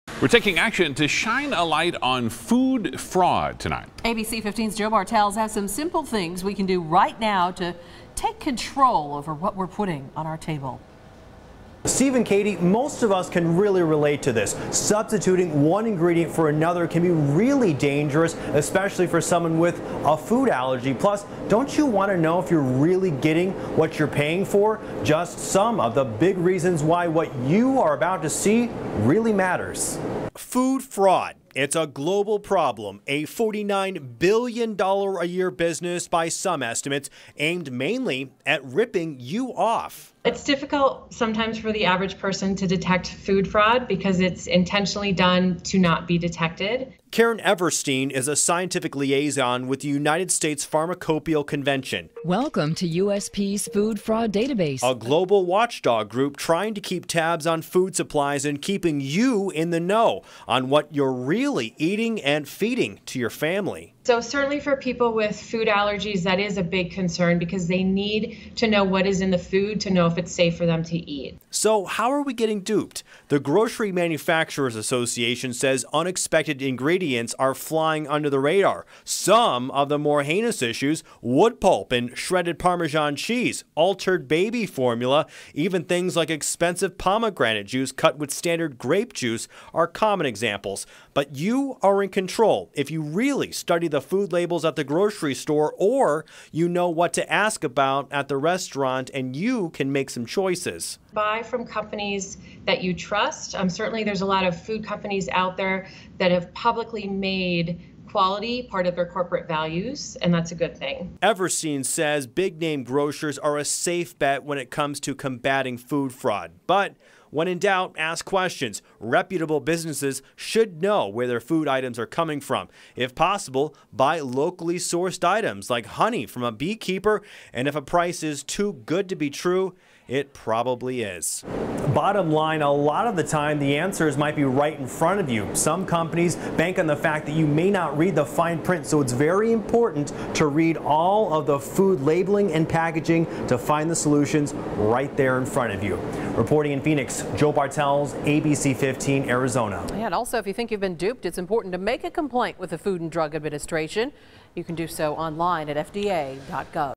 Food fraud: Take control of what you put in your body
Food fraud is more common than we think. Wood pulp in shredded parmesan cheese? Pomegranate juice mixed with grape juice? Do your researched and don't get fooled!